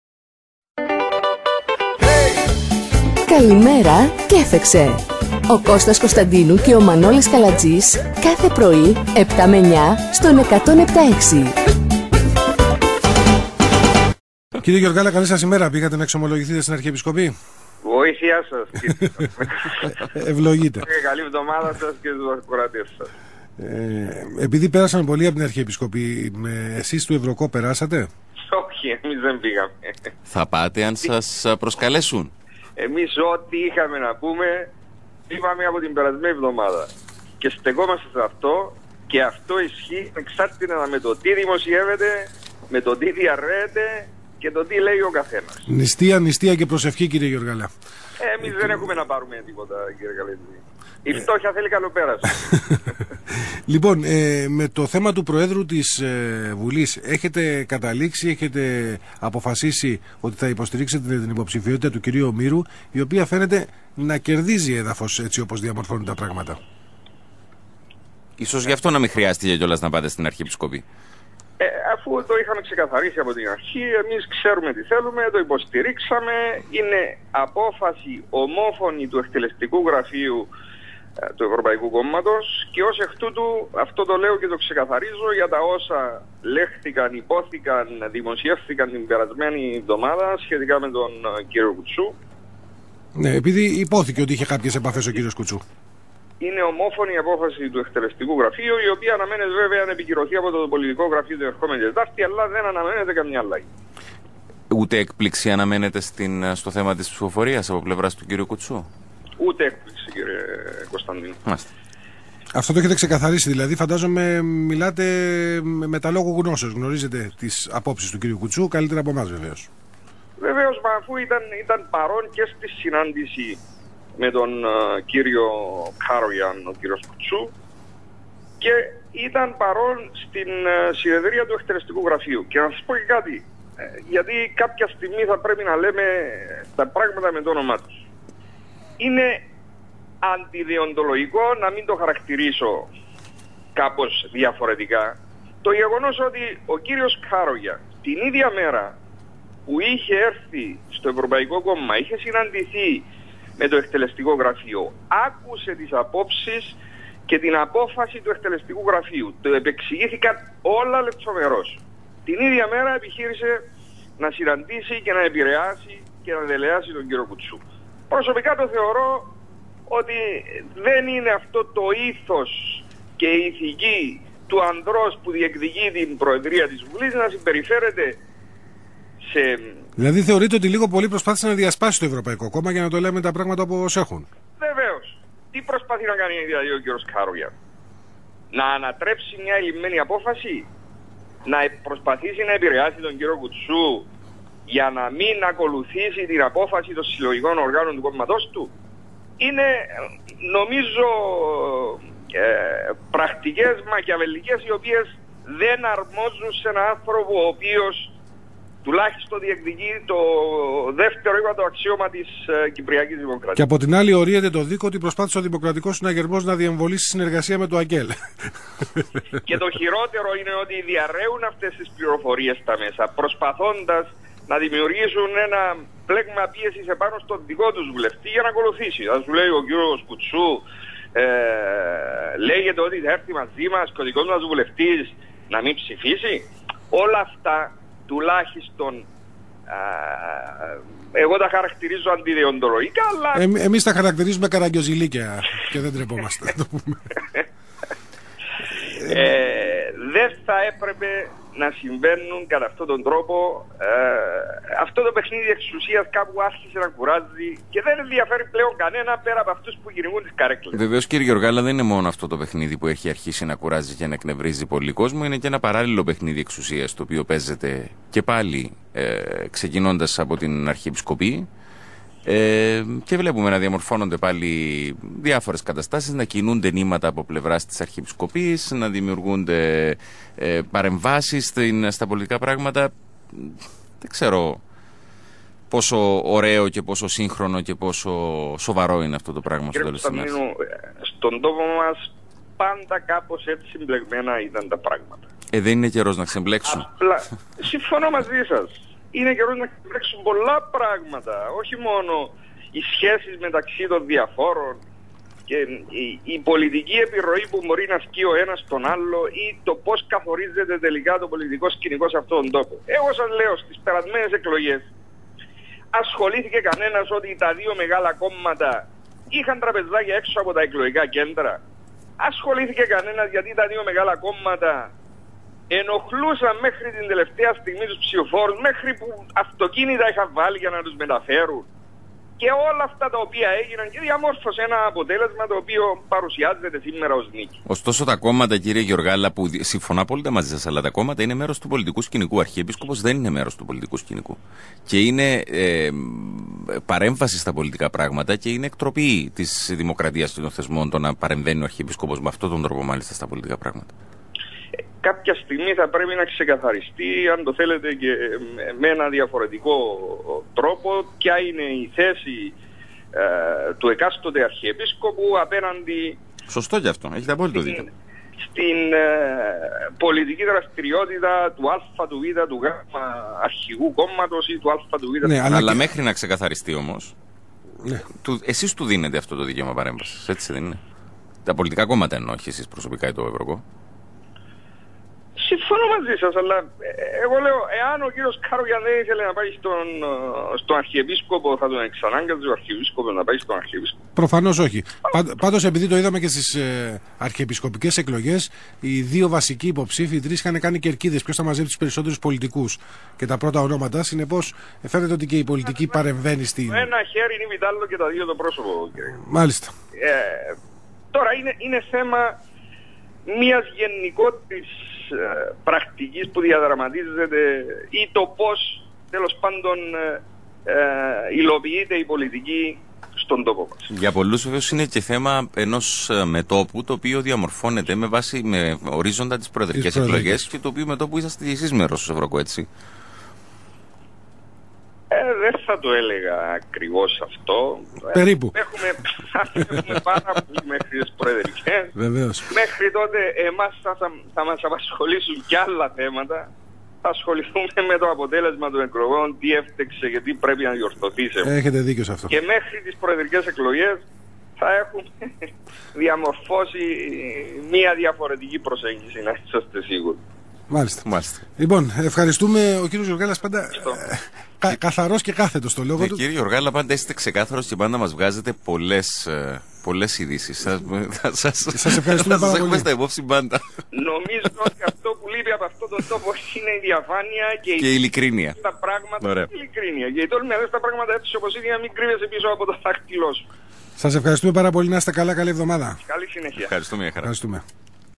Μιχάλης Γεωργάλλας(ΕΥΡΩΚΟ)
Εκτός ηθικής τάξης η παρέμβαση Καρογιάν.